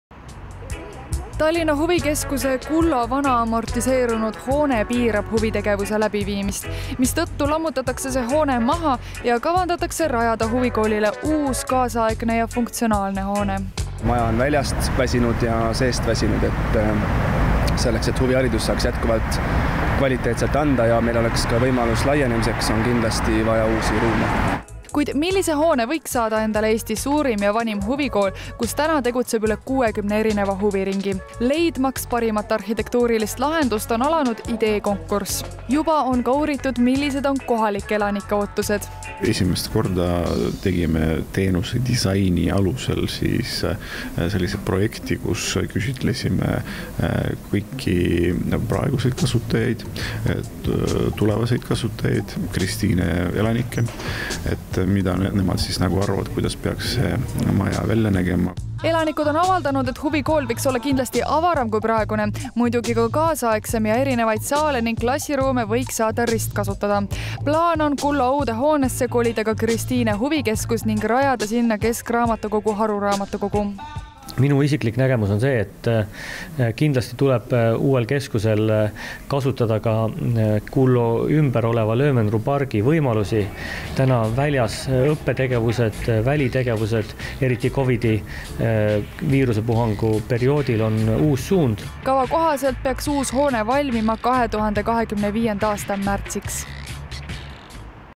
Algas Kullo huvikeskuse uue hoone arhitektuurivõistlus
Tallinna linn kuulutas välja arhitektuurivõistluse Kullo huvikeskuse uuele hoone ja seda ümbritseva Löwenruh pargi ideekavandi leidmiseks. Tallinna linn kuulutas välja arhitektuurivõistluse Kullo huvikeskuse uuele hoone ja seda ümbritseva Löwenruh pargi ideekavandi leidmiseks.